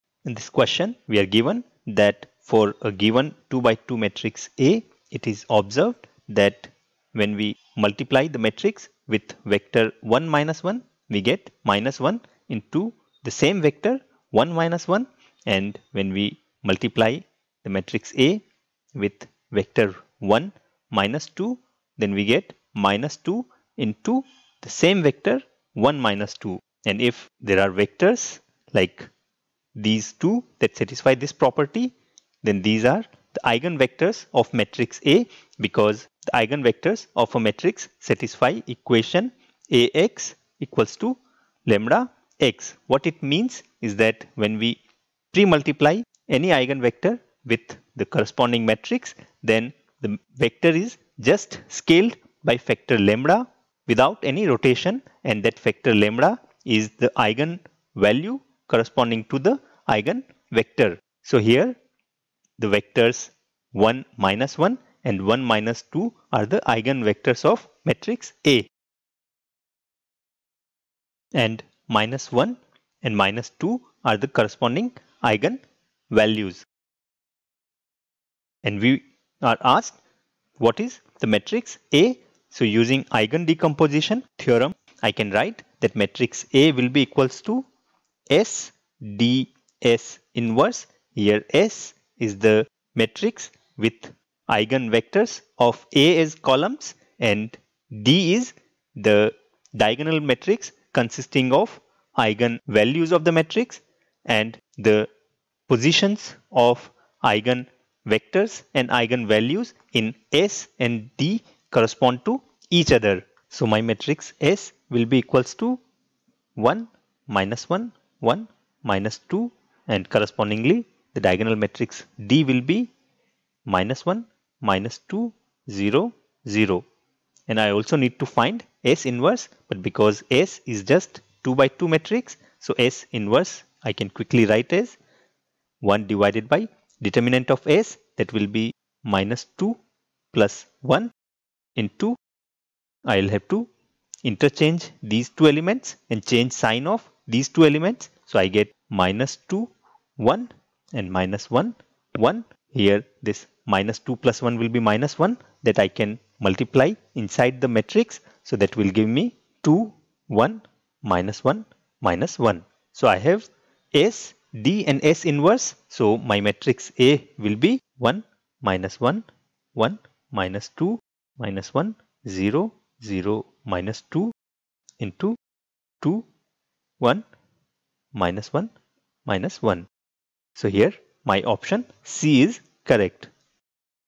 For a given 2x2 matrix A it is observed that GATE IE IN 2006
Theory

Eigenvalue problems often show up in physics and many branches of engineering. Such problems are in the form

A v = lambda * v

Here A is a matrix, v is eigenvector,  lambda is eigenvalue. Eigenvector and eigenvalue are often incorrectly written as eigen vector and eigen value.

The matrix A corresponds to some linear transformation on vector v. The vector v satisfying above equation is called eigenvector.

In general when we multiply a matrix A ( n x n ) with a vector v (n x 1) we get a vector Av, which is not parallel to v. However, when v is an eigenvector of A then A v =  lambda v is parallel to v. lambda  is the factor by which v is scaled, i.e., magnitude of v is multiplied by lambda. lambda  is called the eigenvalue.  v is eigenvector corresponding to eigenvalue lambda .

In GATE most of eigenvalue / eigenvector problems are solved using properties of eigenvalues and eigenvectors.

Property 1. When a matrix is multiplied by its eigenvector then the eigenvector gets scaled by eigenvalue corresponding to the vector without change in the direction ( rotation )  of the eigenvector.

Property 2.  Sum of eigenvalues of a matrix is equal to trace of the matrix, i.e., sum of diagonal elements of the matrix.

Property 3. Product of eigenvalues of a matrix is equal to determinant of the matrix.

Eigendecomposition Theorem

A rectangular matrix can always be written as A = SDS-1 here columns of matrix S are eigenvectors of A and D is a diagonal matrix with corresponding eigenvalues as the diagonal elements.

This part of the lecture covers short cut method to solve eigenvalue and eigenvector problems commonly asked in GATE exam.